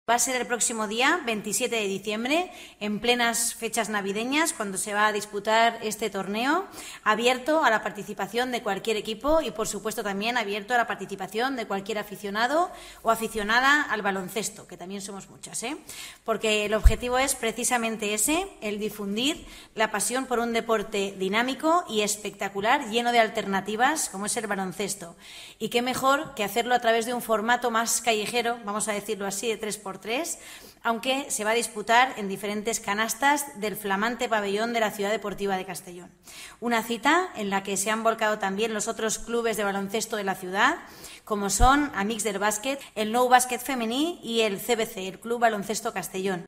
Castellón acogerá el primer torneo ‘3x3’ de baloncesto
La concejala de Deportes, Begoña Carrasco, ha presentado esta mañana el I Torneo 3x3 ‘Domina la Pista’ de baloncesto que organiza el Club Esportiu del Grau y que se celebrará el próximo 27 de diciembre en el pabellón de la Ciudad Deportiva de Castellón.